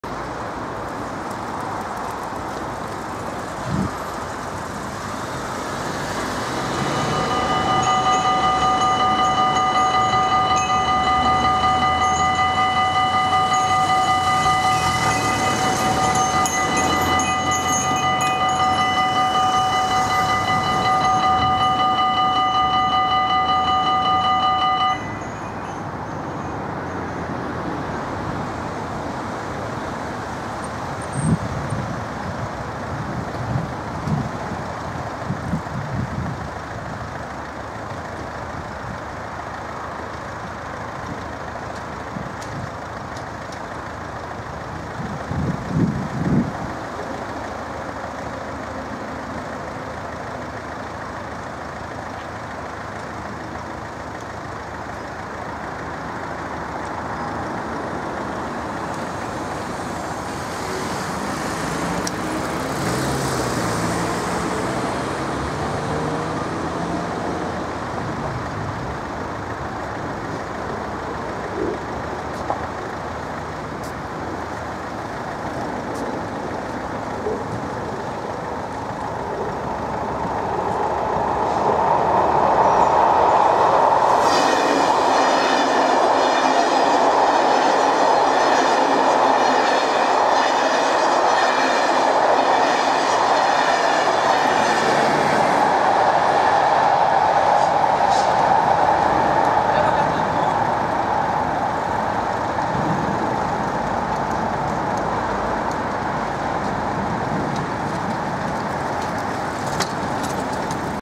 Spoorwegovergang Turnhout/ Passage a Niveau/ Railroad-/ Level Crossing/ Bahnübergang
De IC-trein naar Binche passeert de Oud Strijderslaan

Le Train-IC a destination Binche passe un passage a niveau de Oud Strijderslaan

The IC-Train to Binche passes the Oud Strijderslaan

Der IC-Zug nach Binche fahrt durch die Oud Strijderslaan

d.d. 5 juli 2016